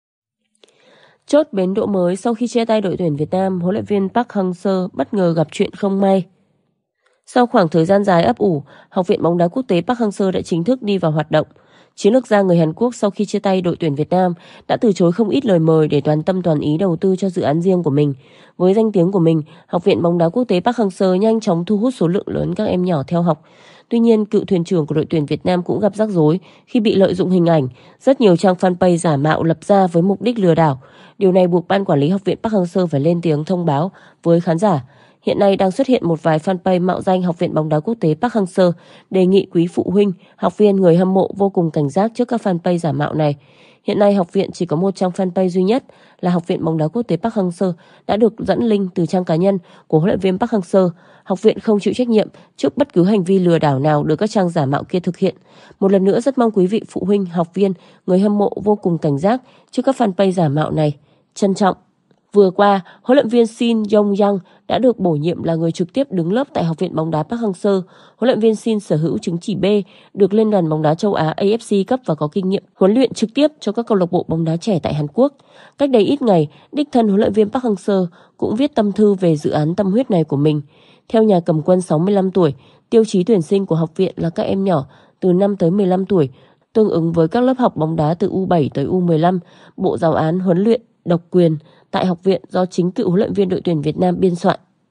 Sau khi chia tay ĐT Việt Nam, HLV Park Hang Seo gặp BIẾN CĂNG
Hy vọng chúng tôi sẽ là nguồn kiến thức hữu ích mỗi ngày cho quý vị và các bạn